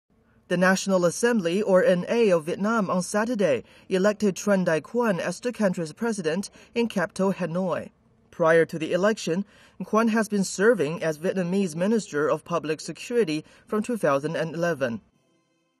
Vietnamese public security minister elected as new Presi
The National Assembly (NA) of Vietnam on Saturday elected Tran Dai Quang as the country's president in capital Hanoi.